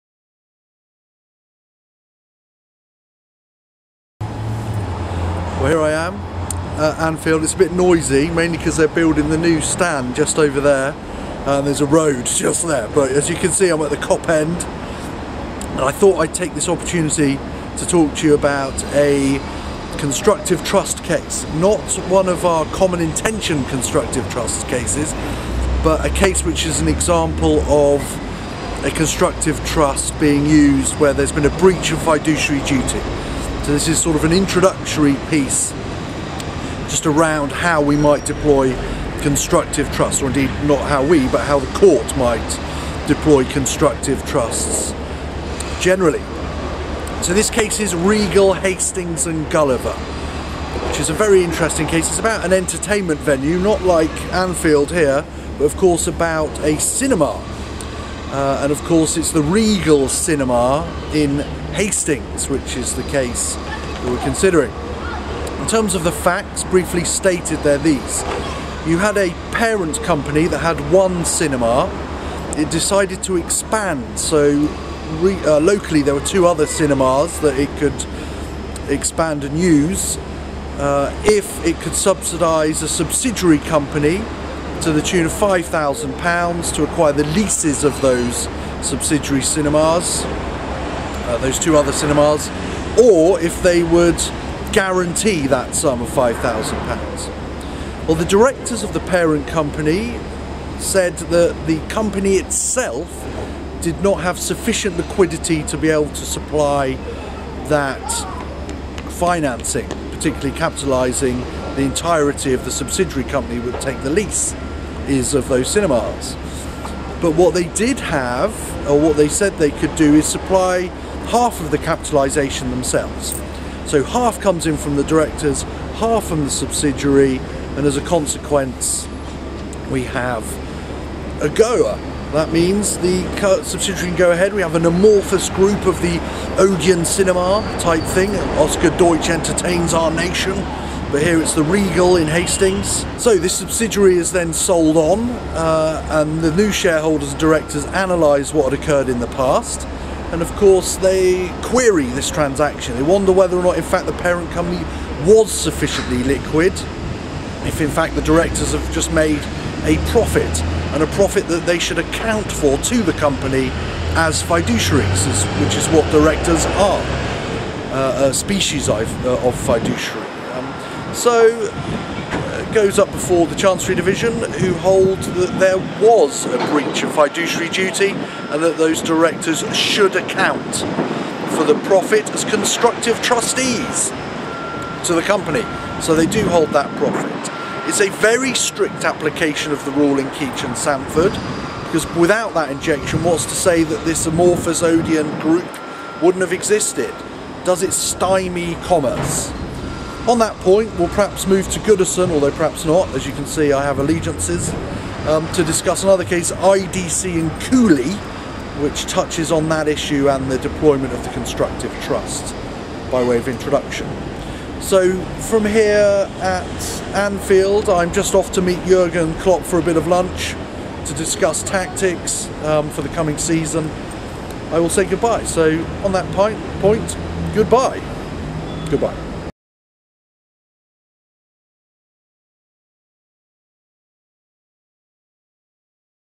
Equity Short: Constructive Trusts and breach of fiduciary duty - Regal Hastings v. Gulliver
Filmed on location outside Anfield Football Stadium, this “Equity Short” features Dr. Tribe discussing this key authority on the deployment of constructive trusts in the context of a breach of a fiduciary duty owed to a company. Dr. Tribe demonstrates how the diversion of a potential corporate opportunity could give rise to the use of a constructive trust.